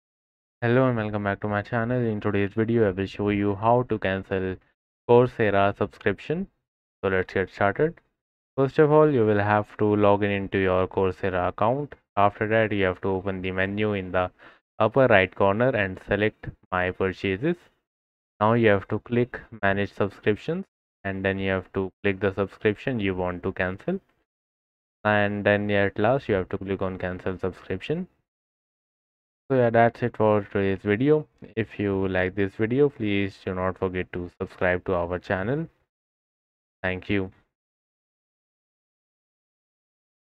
✅ How To Cancel Coursera Subscription (Full Guide)
Learn how to cancel your Coursera subscription with this easy step-by-step guide. We’ll show you how to manage your Coursera account and avoid future charges.

Key Learning Points:
• Canceling Coursera subscriptions
• Step-by-step guide for managing Coursera account
• Avoiding future charges on Coursera